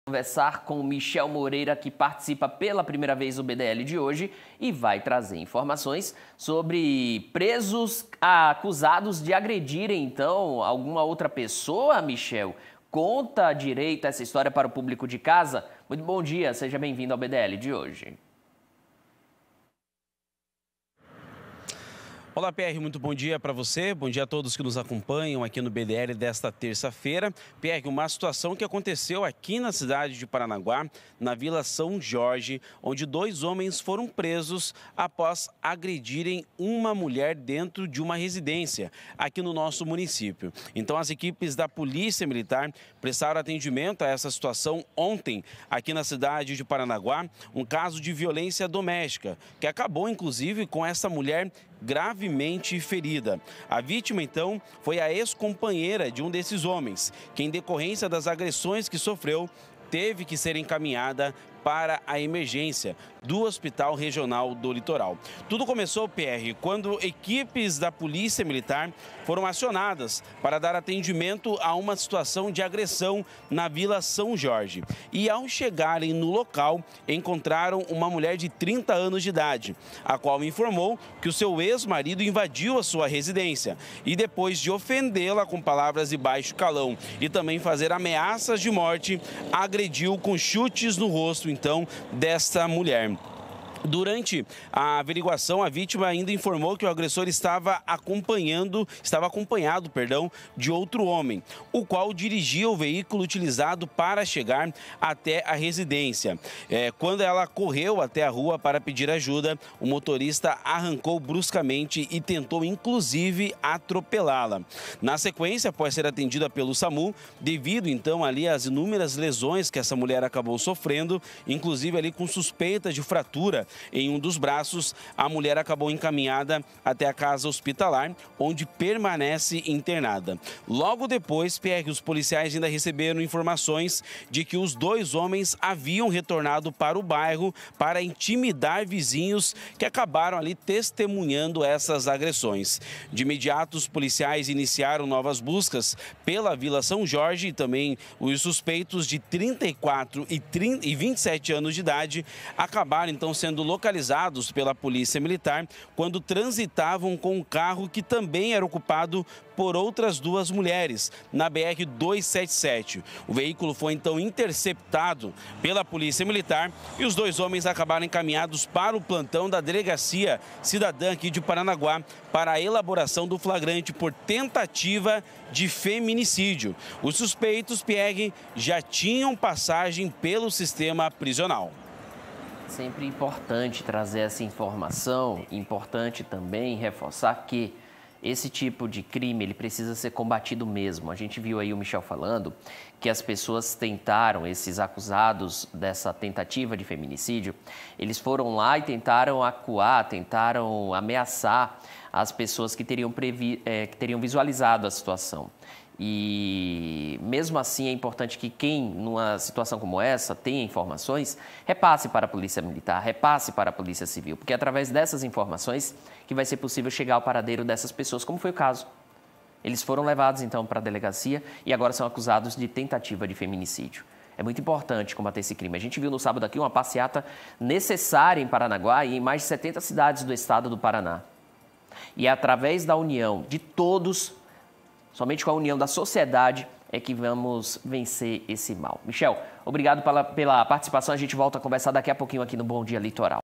SITUAÇÃO NA VILA SÃO JORGE
Dois homens foram presos após mulher ser agredida em Paranaguá